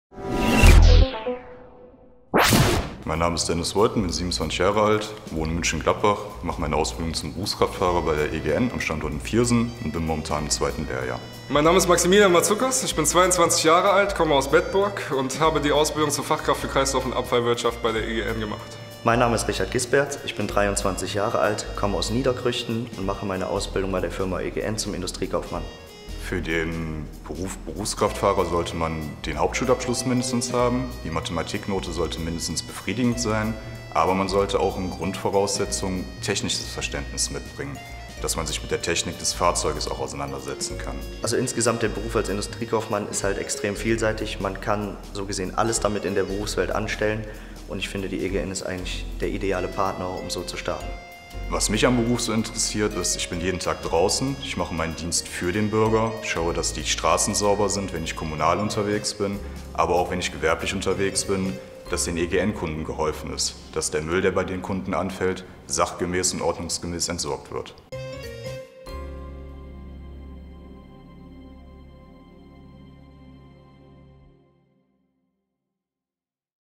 Statementfilm der Entsorgungsgesellschaft Niederrhein mbh (EGN)
Die EGN stellt ihre Ausbildungsberufe vor. Interview mit drei Azubis, die aus ihrem Arbeitsalltag berichten.